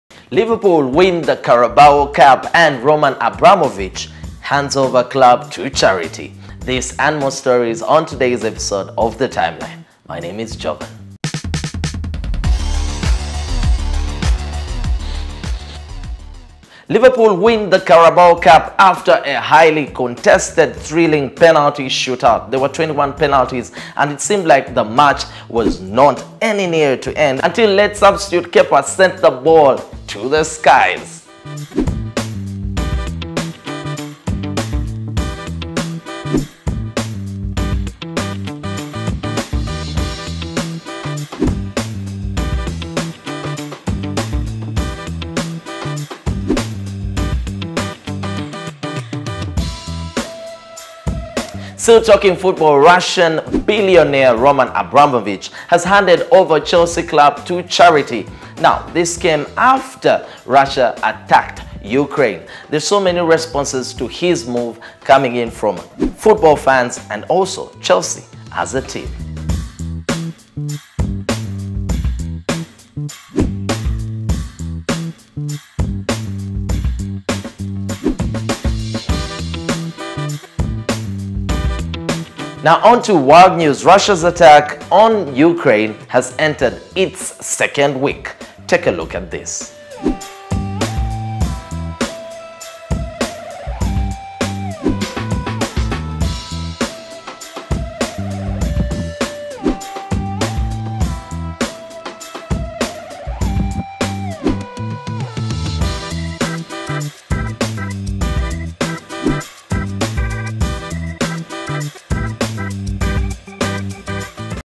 The timeline256 : RUSSIA - UKRAINE
Russia continue to strike Ukraine amidst reactions from across the world. Liverpool wins the Carabao cup and Roman Abramovich takes a back sit at Chelsea